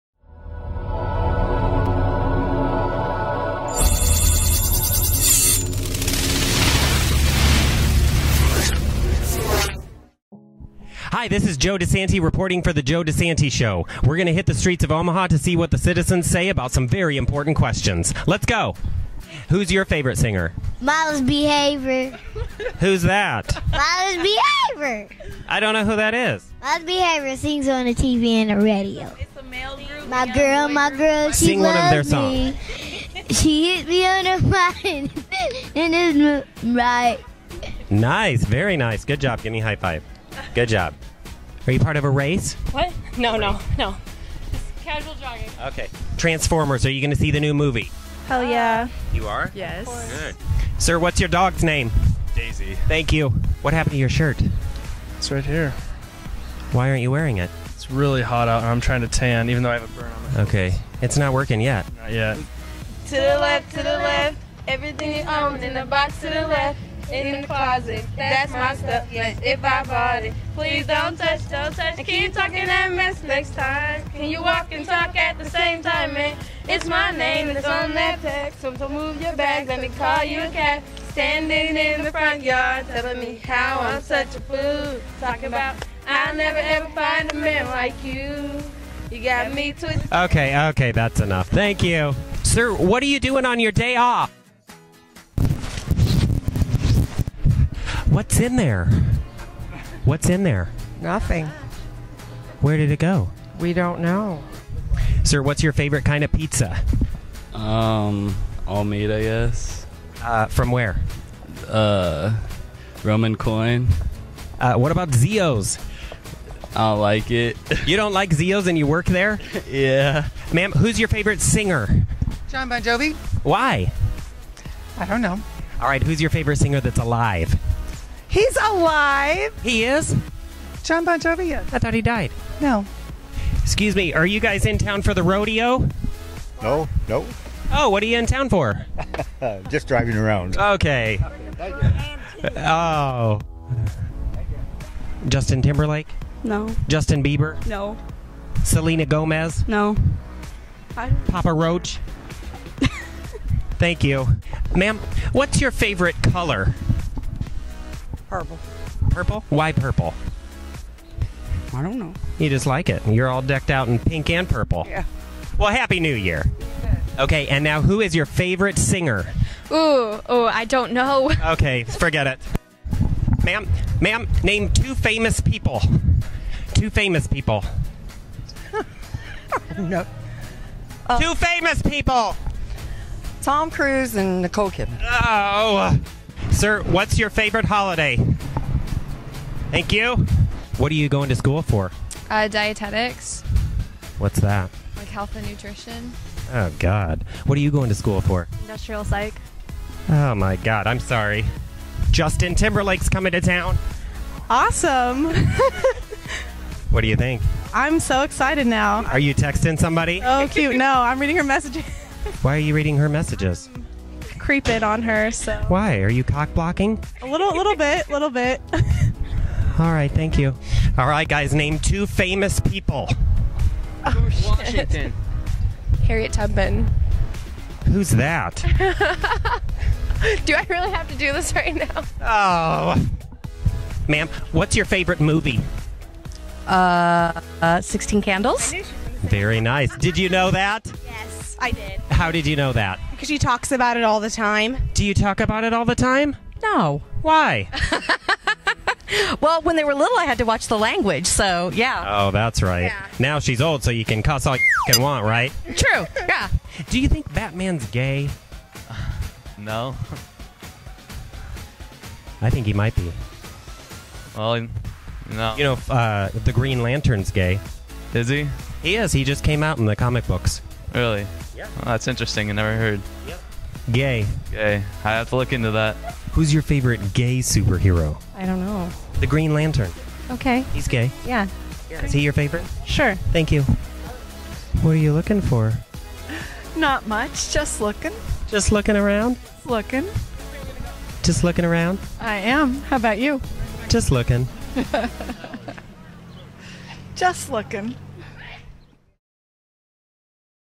The Joe DeSanti Show - Man On The Street - Episode 1
Joe hits the streets of downtown Omaha to ask the important questions.